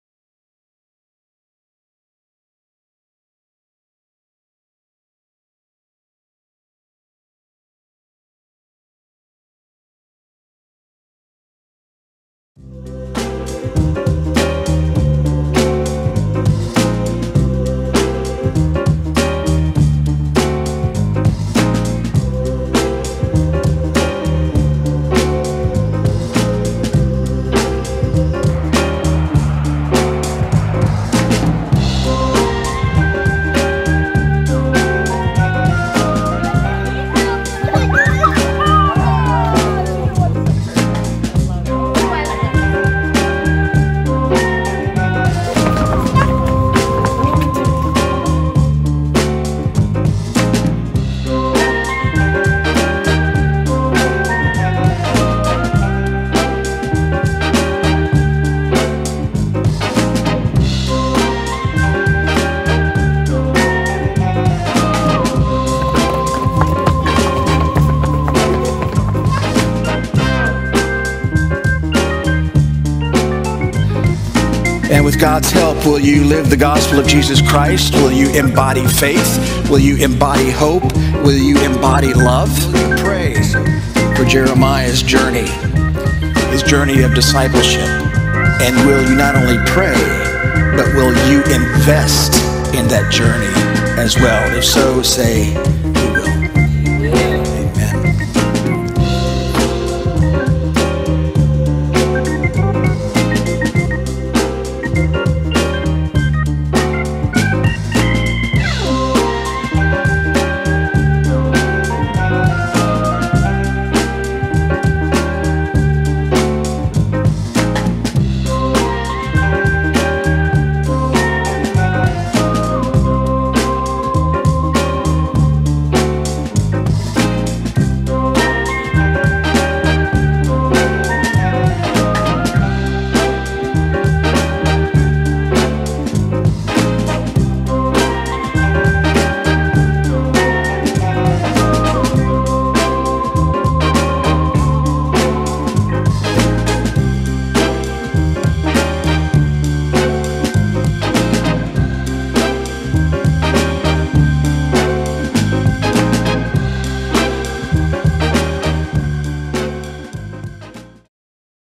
"On the Block, For the Block: Our Kids" Annual Giving Initiative video
Learn how Park Avenue Church is reaching out to our neighborhood through our 2022 Giving Initiative.